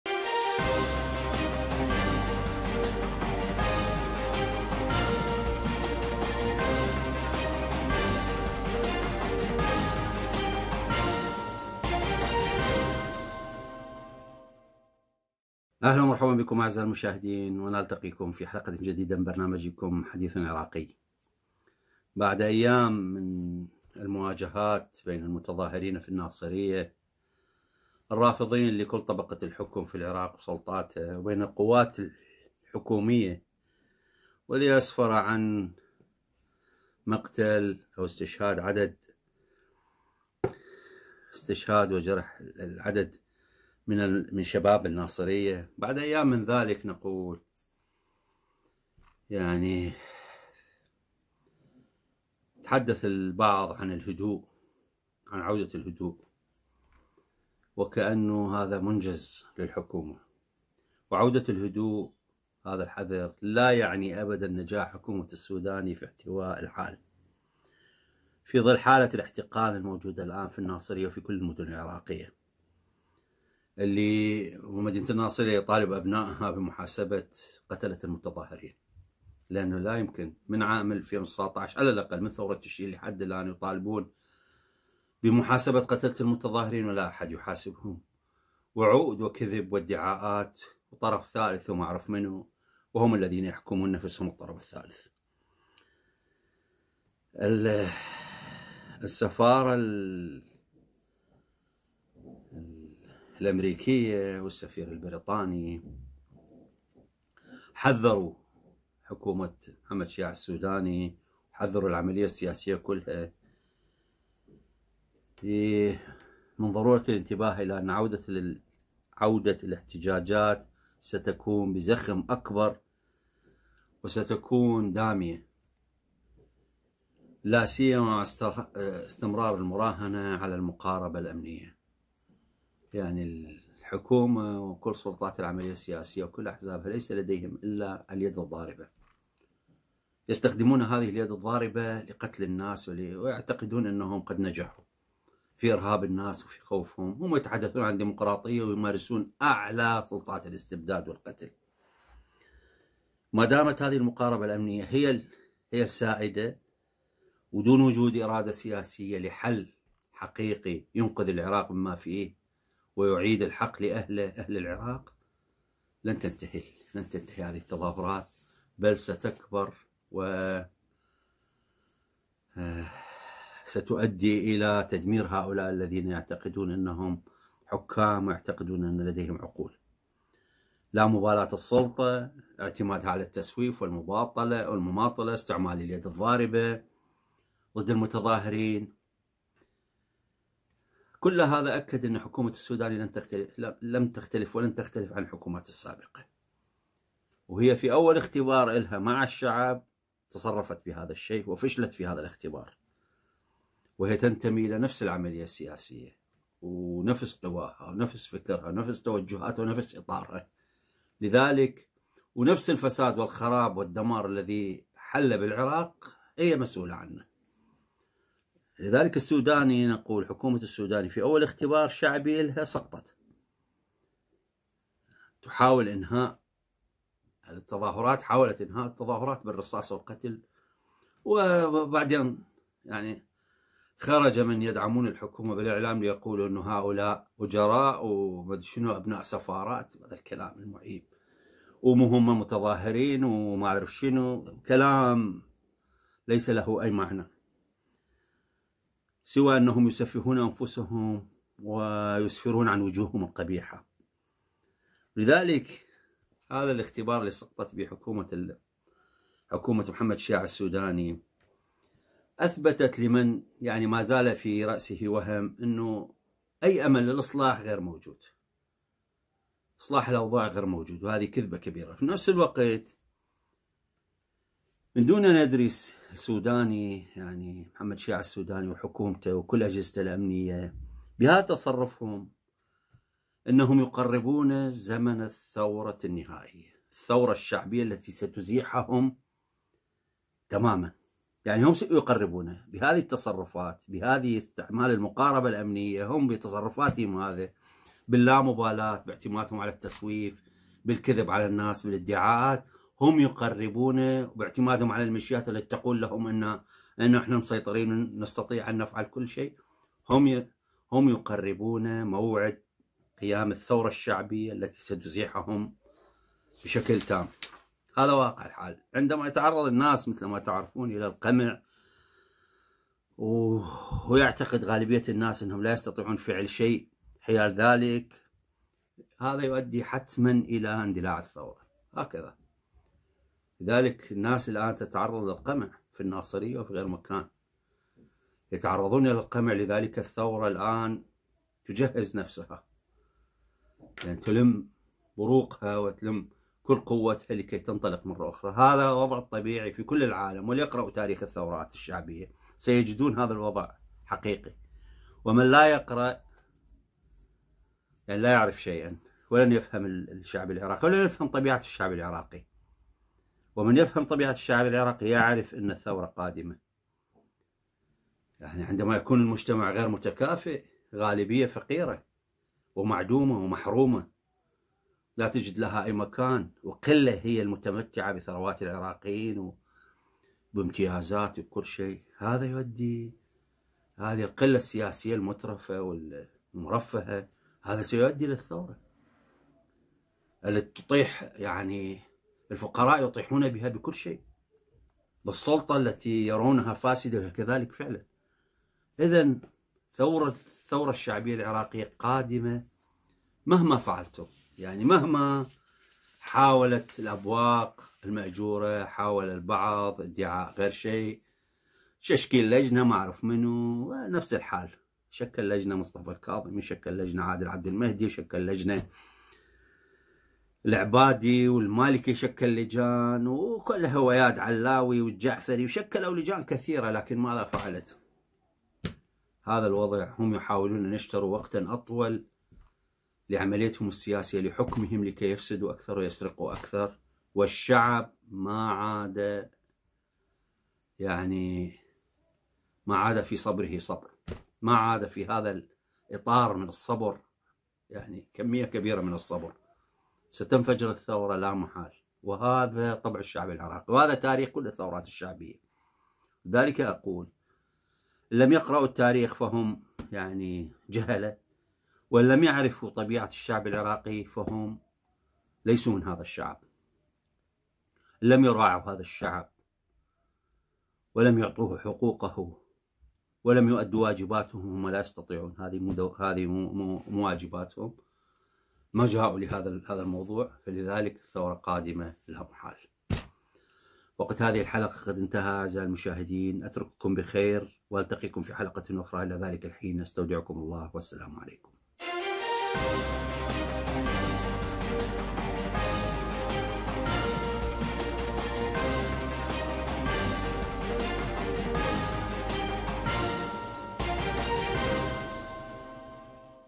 حديث_عراقي | هل الثورة الشعبية العراقية قادمة؟
تقديم الدكتور رافع الفلاحي